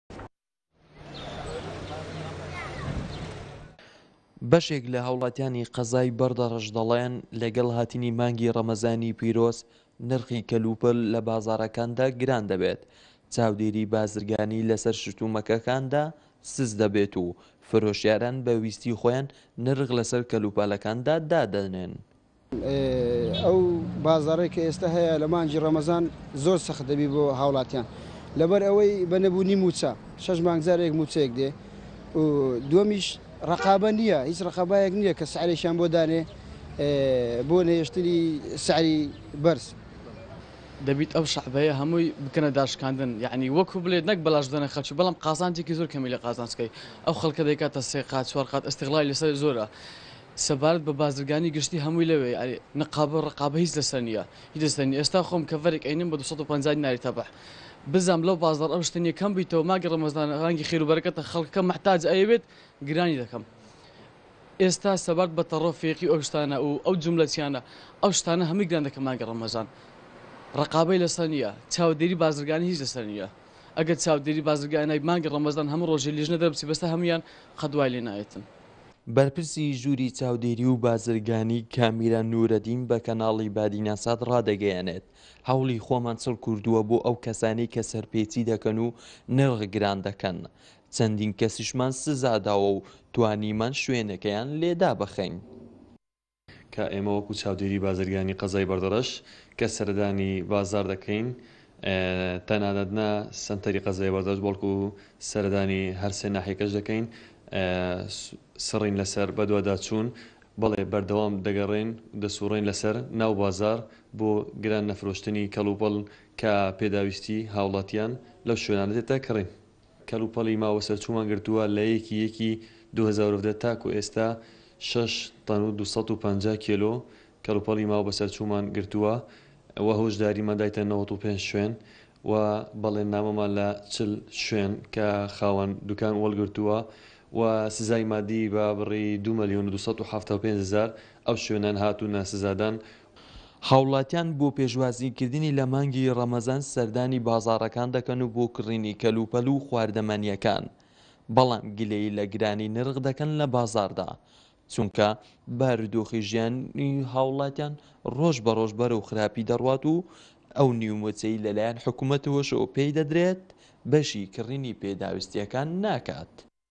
bardarash
بەردەرەش...وەلاتی گازندێ ژ گرانبوونا بھایان دکەن